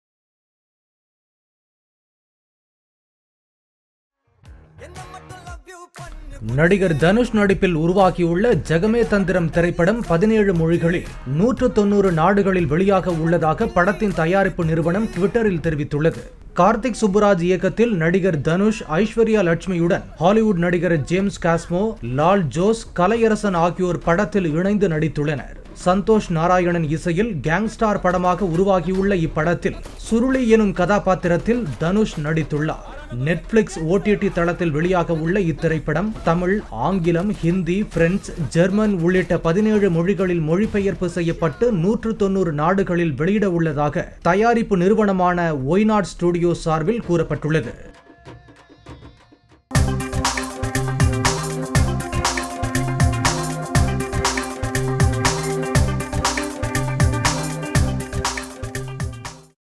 17 மொழிகளில் 190 நாடுகளில் வெளியாகிறது தனுசின் 'ஜகமே தந்திரம்' | Jagame thandiram
சின்னத் திரையின் சினிமா இனிமே_இப்படித்தான் 17 மொழிகளில் 190 நாடுகளில் வெளியாகிறது தனுசின் 'ஜகமே தந்திரம்'..!